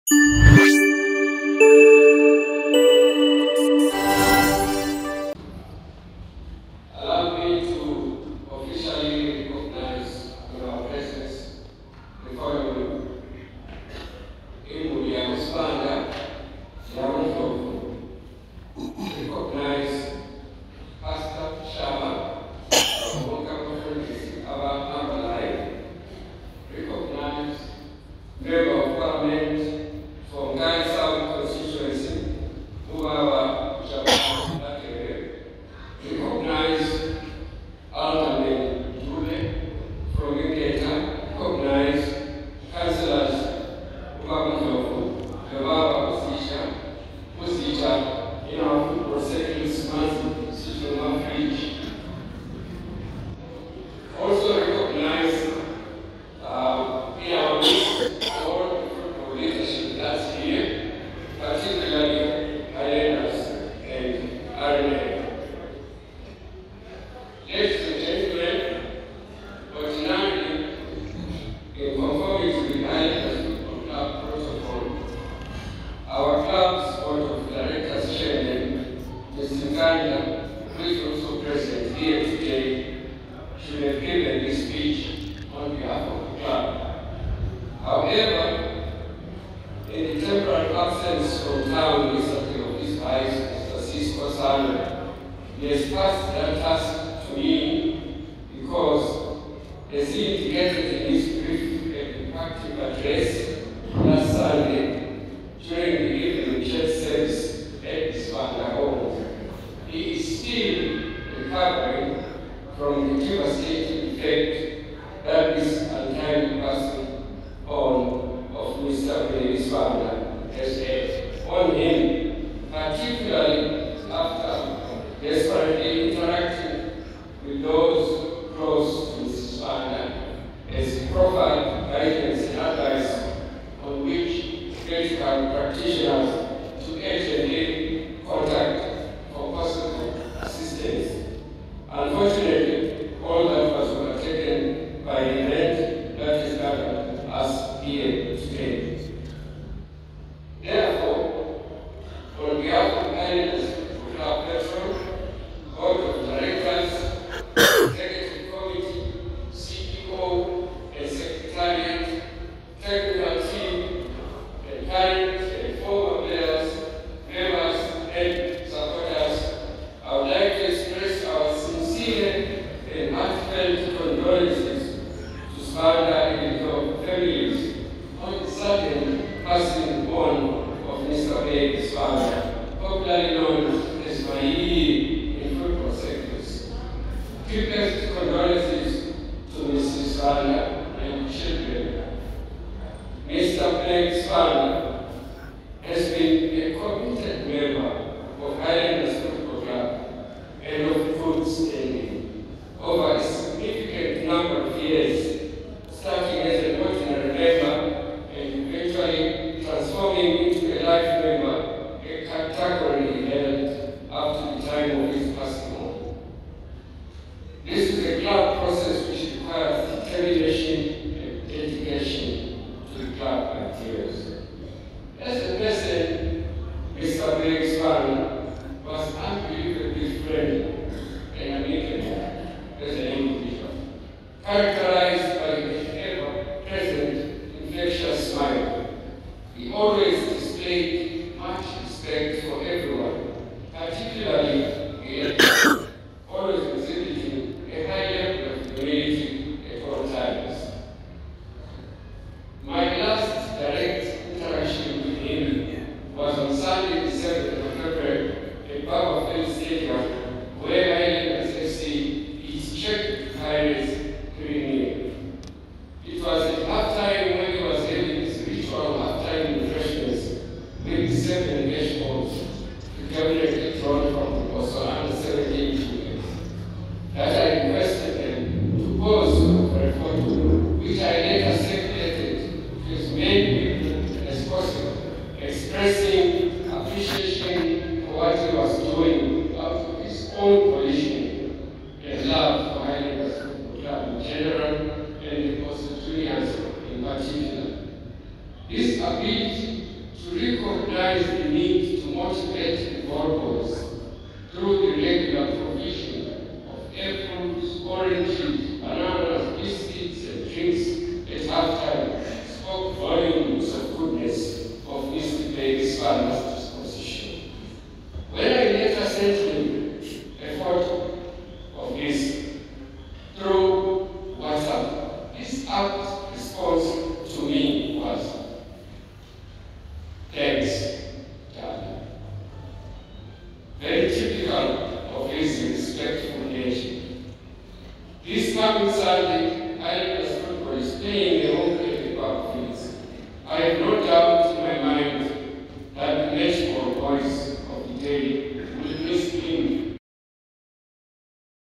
Bosso bids Bheka Sibanda farewell
Highlanders FC board member Elkanah Dube reads  the team's speech at the former club member's funeral service at Bulawayo Amphitheatre.